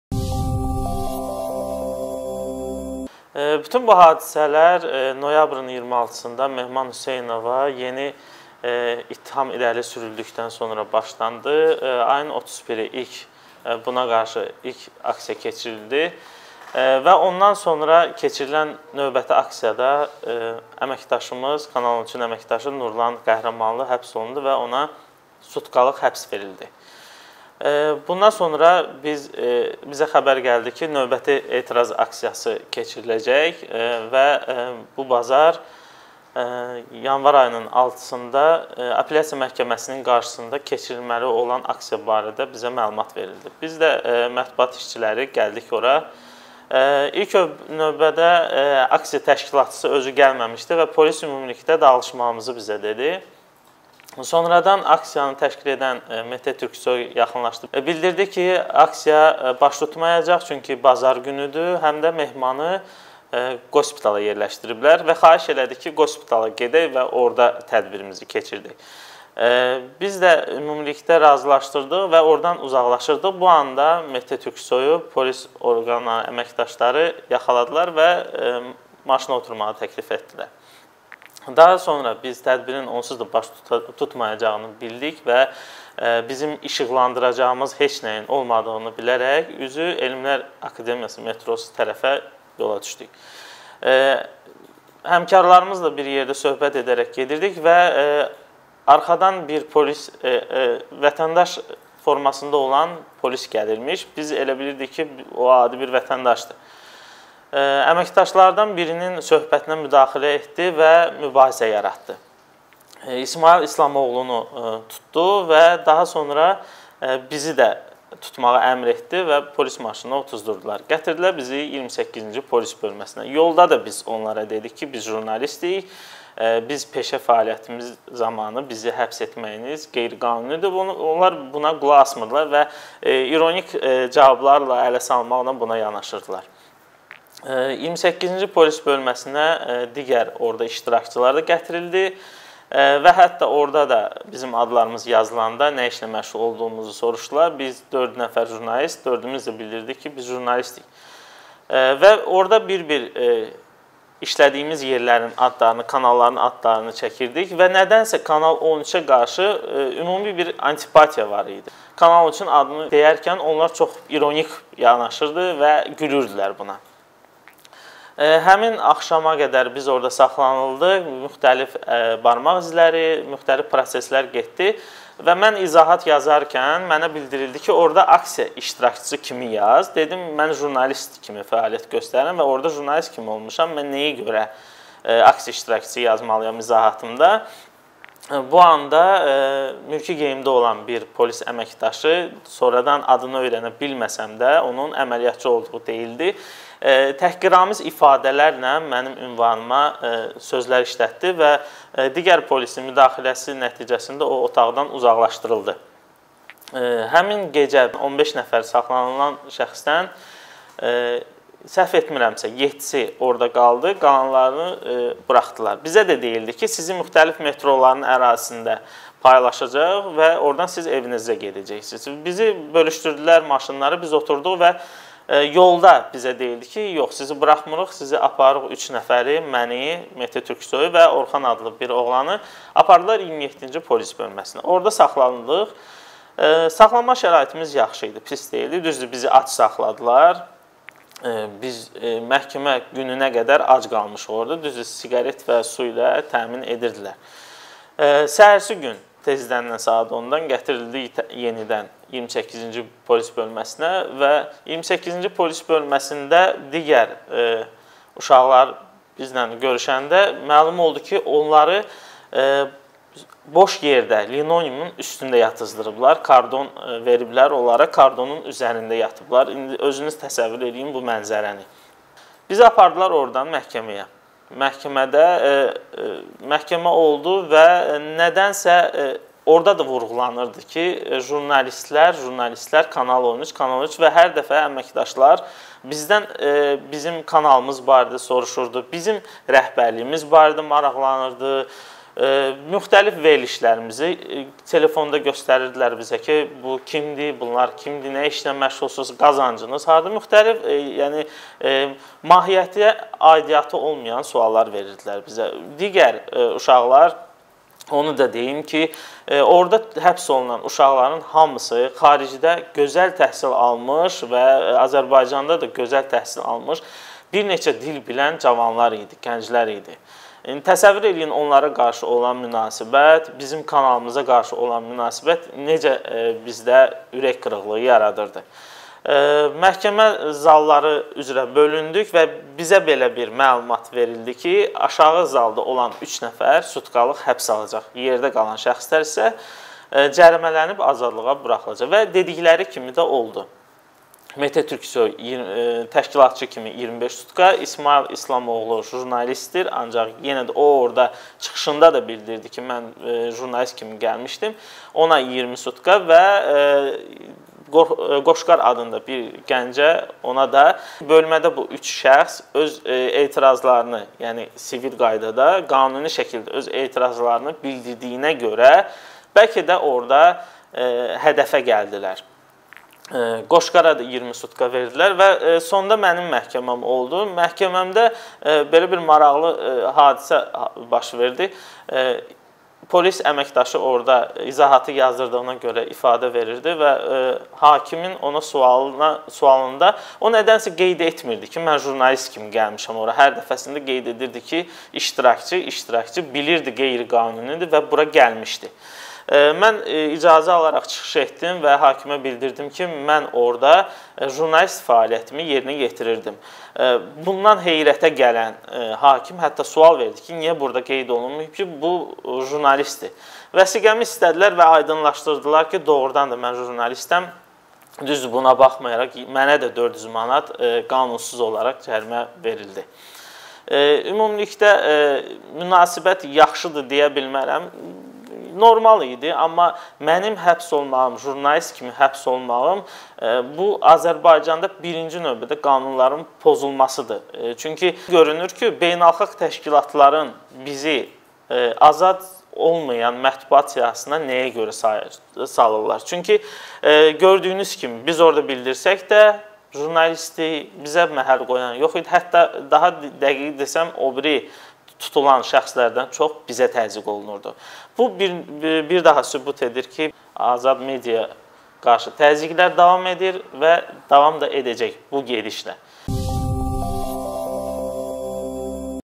"Polislər Kanal13-ə xüsusi antipatiya ilə yanaşır, bizə adi iştirakçılardan daha çox təzyiq var idi"
"Azad mediaya qarşı basqılar davam edəcək. Saxlaninanlardan bəziləri quru yerdə gecələməli olmuşdu. Heç bir yemək verilmirdi. Sadəcə siqarət və su verilirdi. Məhkəməyə ac gətirildik" - Elçin Rəhimzadə, Kanal13-ün əməkdaşı